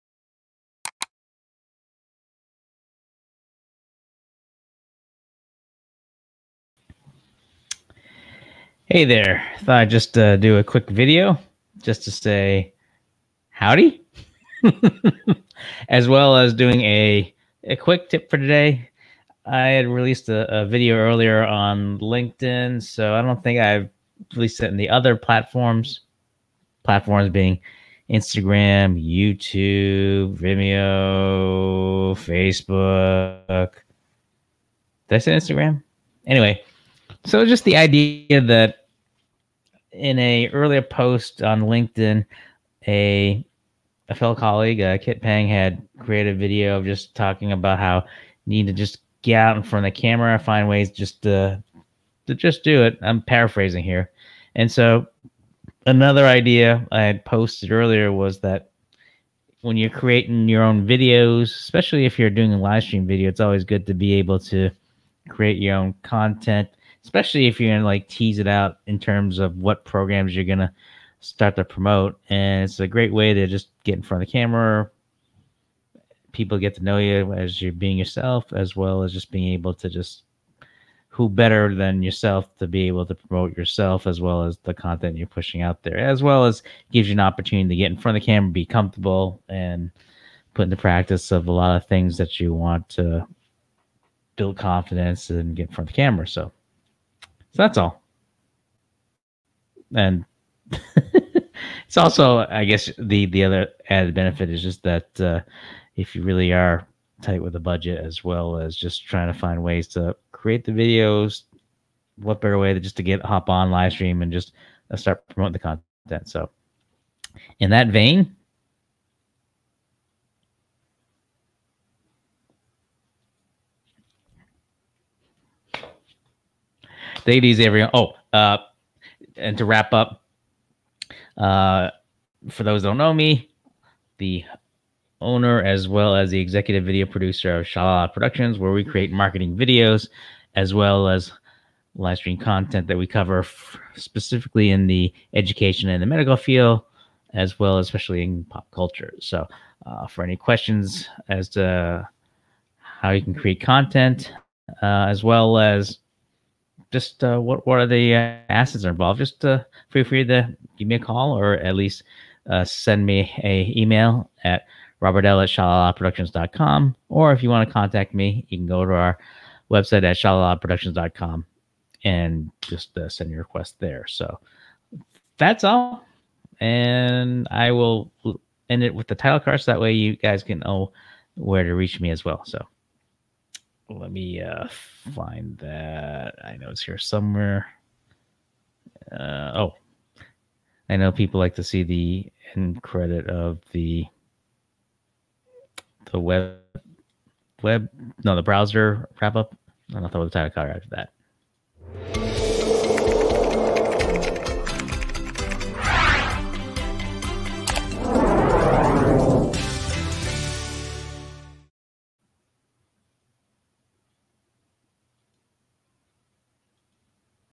ShaLaLaLatips4today video marketing workflow tip and a teaser for Episode 4 for Verbosewednesday
(video marketing workflow tip & a teaser for Episode 4 for Verbosewednesday)

Workflow Tip
Here's a tip I shared with Kit Pang 🎤 (on his chat) as he was on LinkedinLive earlier about adding to his point of finding ways to getting comfortable in front of the camera. Use Livestream to your advantage and post your own teasers, or videos in promoting your own event or even as a response to a post you are following. It's the best way of showing your authentic self, (as Bob Cargill preaches in his post on Twitter, LinkedIn, YouTube, Facebook, and other social media platform)

For those pressed for time, limited budget, and need to create videos to promote an event or scheduled content i.e. live streaming videos, the best way to "crank it out" is to go on live, and promote the event. Who better knows the event or content better than yourself? No one. Get comfortable and get in front of the camera that Kit Pang 🎤 pointed out in his linkedinlive video. The more times you do it the comfortable you'll be. It'll take practice but you'll get there, but also as Chris Weiher - CLEAVER Creative preaches, don't forget the Close Caption/titles. :)

Here's an example that I did in promoting our weekly livestream show verbosewednesdays.